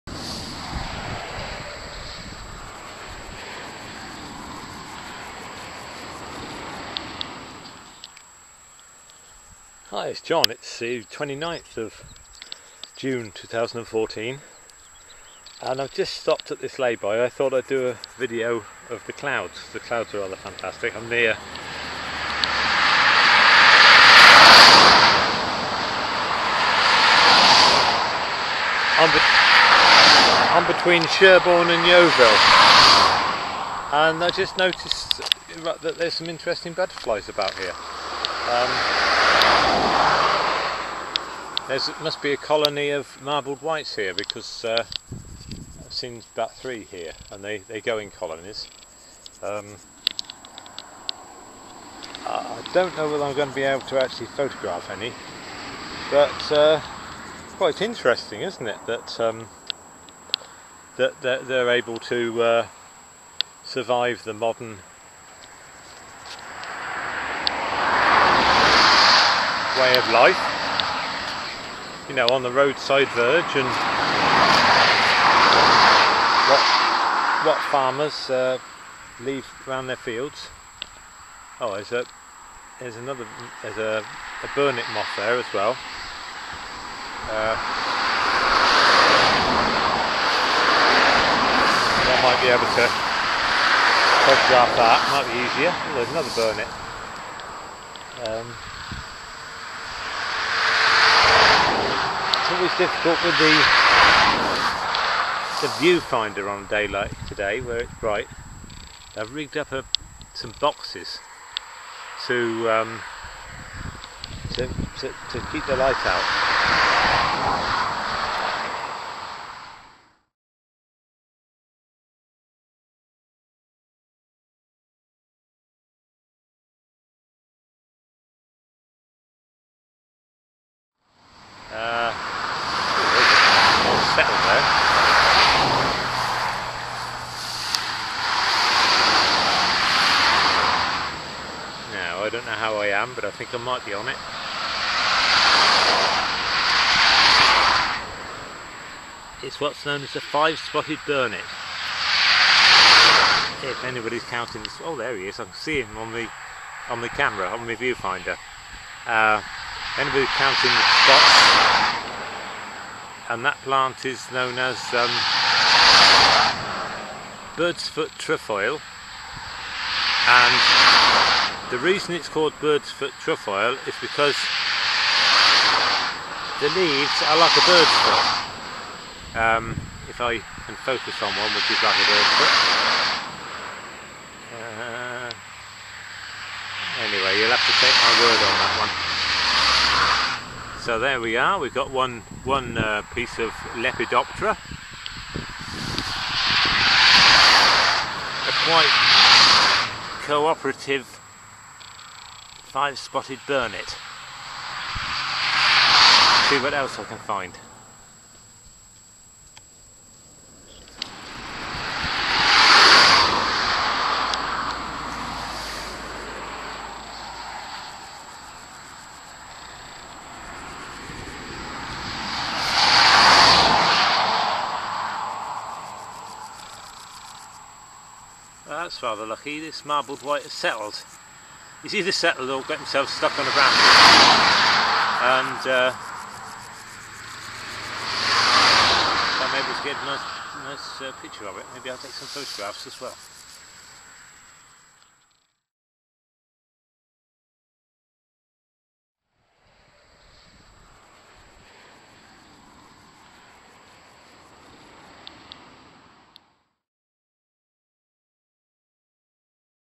Colony of Marbled White Butterflies in the road verge 29/6/14 Melanargia galathea
Colony of Marbled White Butterflies in the road verge 29/6/14

The Video was taken in a lay-by on the A30 about a mile out of Sherborne, Dorset. The creatures must know not to get too close to the traffic, and the grass cutting regime must suit their life cycle, I think the caterpillars feed on a certain sort of grass, and the eggs are scattered around..